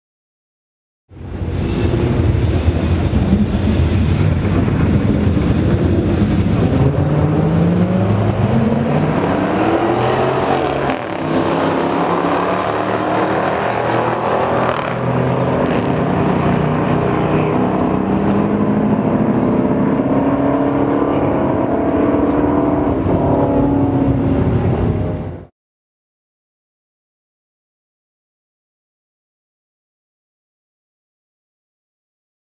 2006 Dodge Charger R/T Vs. 2012 Dodge Chrager R/T | Drag Race
Different model years of the same car, a Dodge Charger, battle it off to see who reigns supreme.

Filmed at Autoclub Speedway in Fontana, CA on 11/18/15

Also.